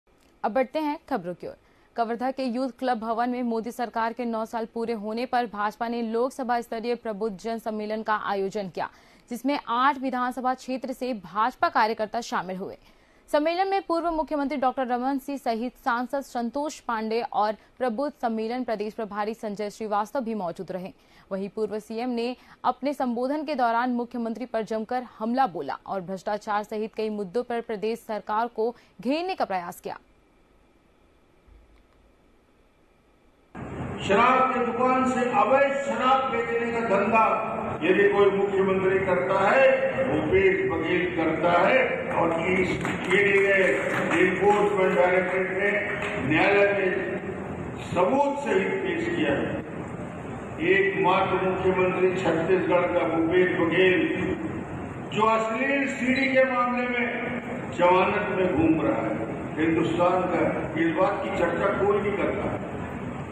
Raman Singh ने Cm Bhupesh ऊपर बोला हमला, कहा भ्रष्टाचार कहा में डूबी प्रदेश सरकार |TV24 MP/CG NEWS
Video Tilte :-Raman Singh ने Cm Bhupesh ऊपर बोला हमला, कहा भ्रष्टाचार कहा में डूबी प्रदेश सरकार |TV24 MP/CG NEWS